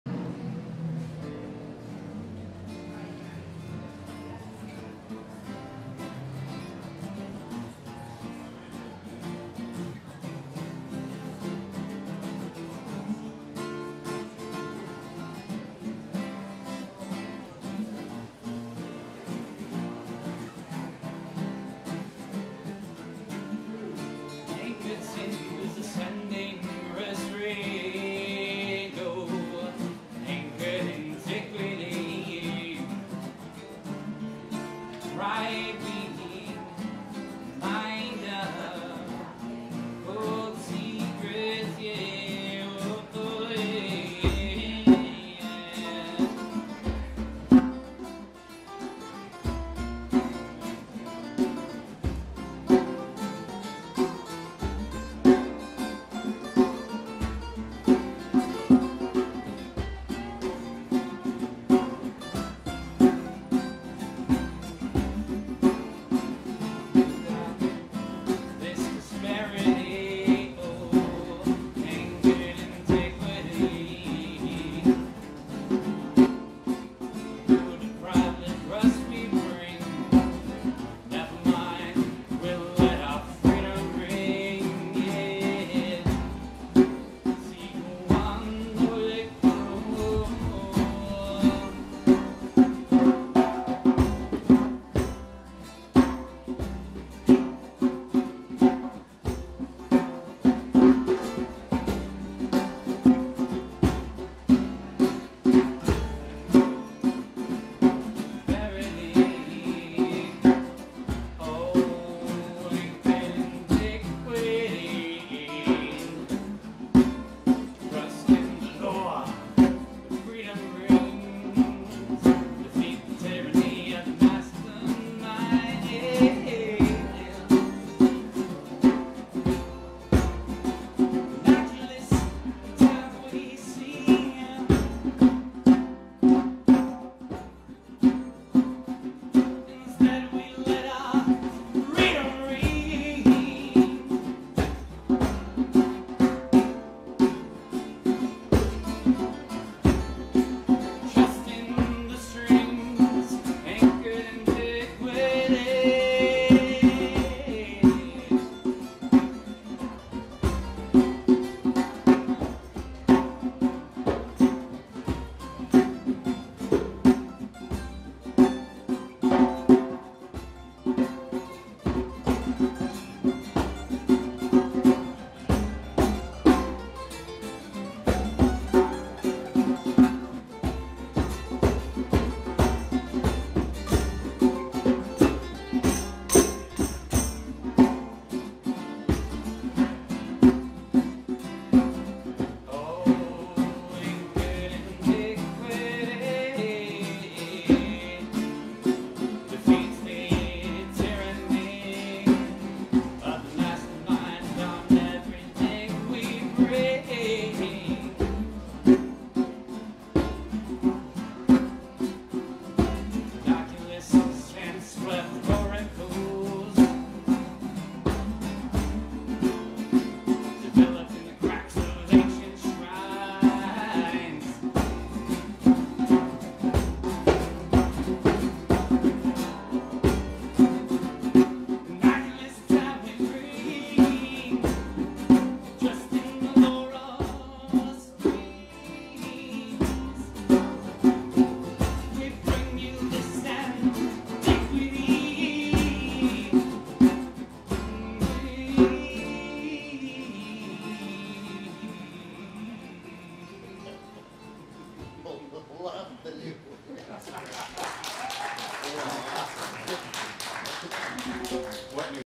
antiquity 8/25/24
Redline Alchemy acoustic set August 25th 2024. they havent played together for over 5 years but they came together on stage with no practice together